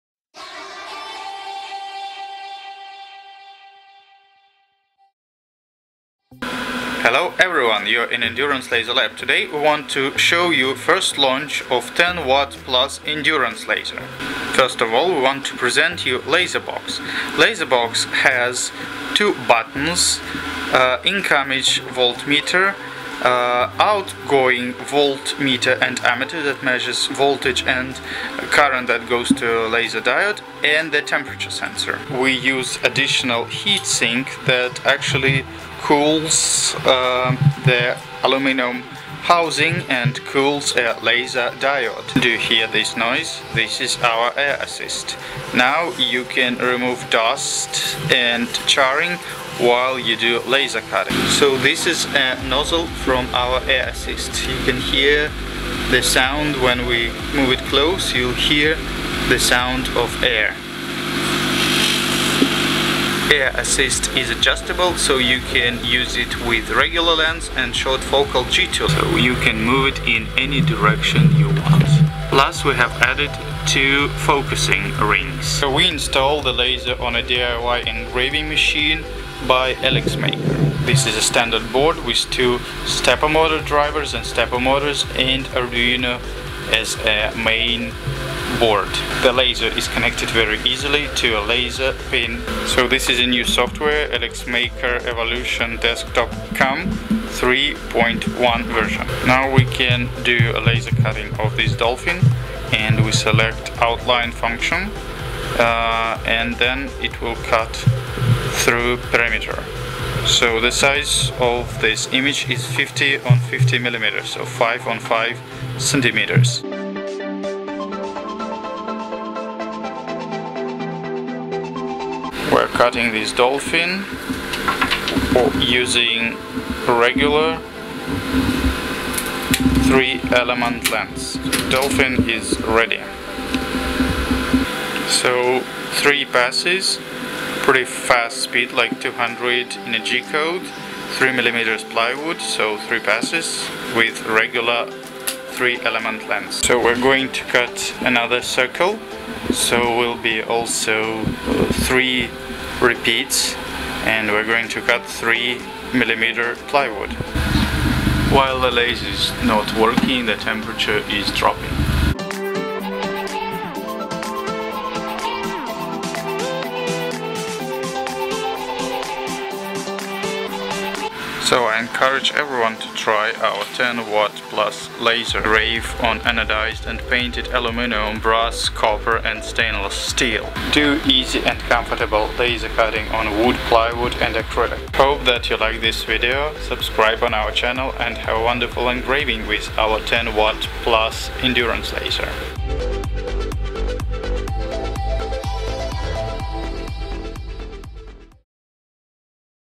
An Endurance 10 watt+ (plus) laser installed on a Eleksmaker DIY machine
First launch of an Endurance 10 watt laser+ installed on Eleksmaker machine.
Do some engravings and cuttings.

Very powerful and very reliable